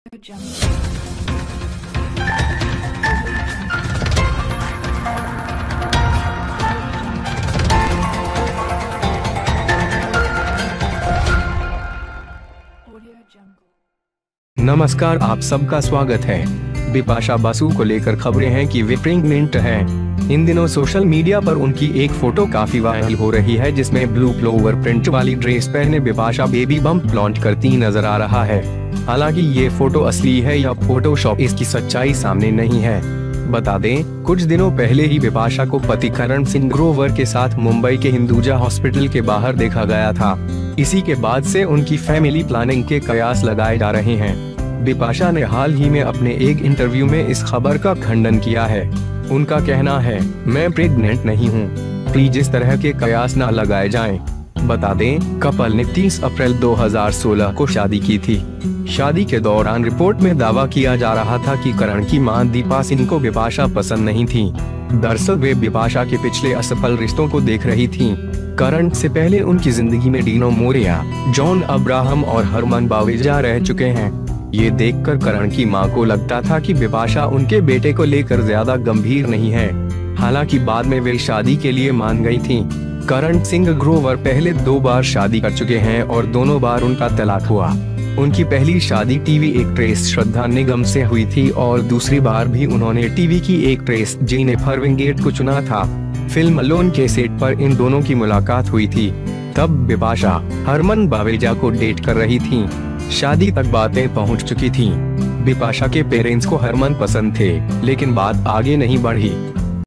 क्या प्रेग्नेंट हैं बिपाशा बसु?, वायरल हुई बेबी बंप फ्लॉन्ट करते हुए PHOTO | Bipasha Basu
There are reports about Bipasha Basu that she is a premanent. These days, one of his photos on social media is getting viral, with Bipasha Baby being a bump flutter wearing Blue Flower printed dress. Although these photos are real or photoshop, its truth is not in front. Let me tell you, Bipasha was seen out of Hinduja Hospital in Mumbai with Karan Singh Grover, a few days back. After this, the spec plans for their family planning are being implemented.
Bipasha has recently denied this news in one of her interviews. - They say, "I am not pregnant. Please do not let this kind of speculation." - Let me tell you, Kapal had married on April 30, 2016.
During the marriage, the report was being claimed that Karan's mother Deepa Singh did not like Bipasha. - Actually he was looking at Bipasha's last unsuccessful relationship. Prior to Karan, Dino Morea, John Abraham and Harman Baweja have lived in their life.